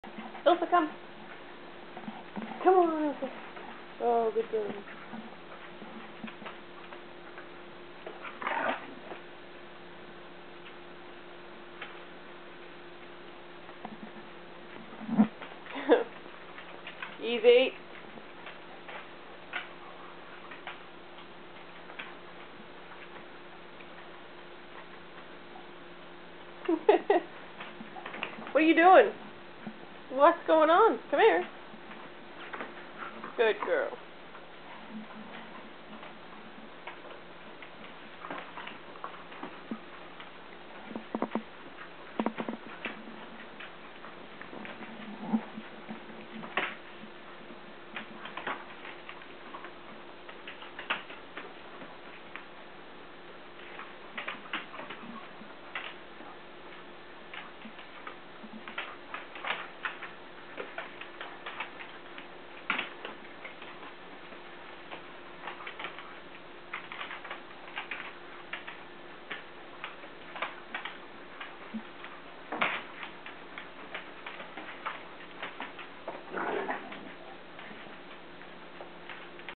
Ailsa and Robbie Playing
Robbie being a pest until Ailsa finally plays with him... until the fun police arrives.